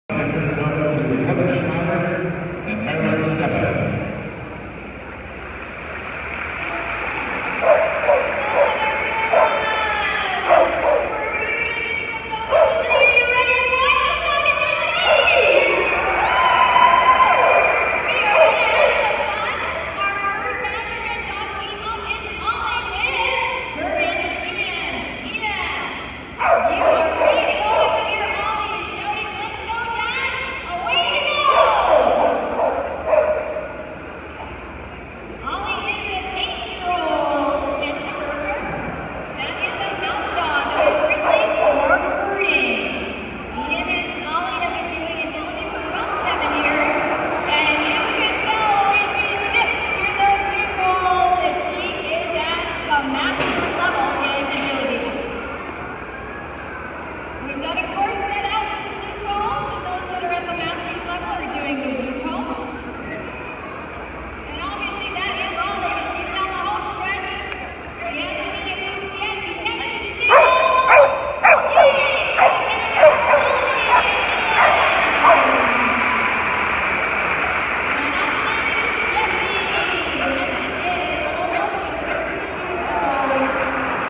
Olie and Ian RCMP Musical Ride demo 8-9-18
CODAC Dog demo at RCMP Musical Ride Show 8-9-18 Kelowna, BC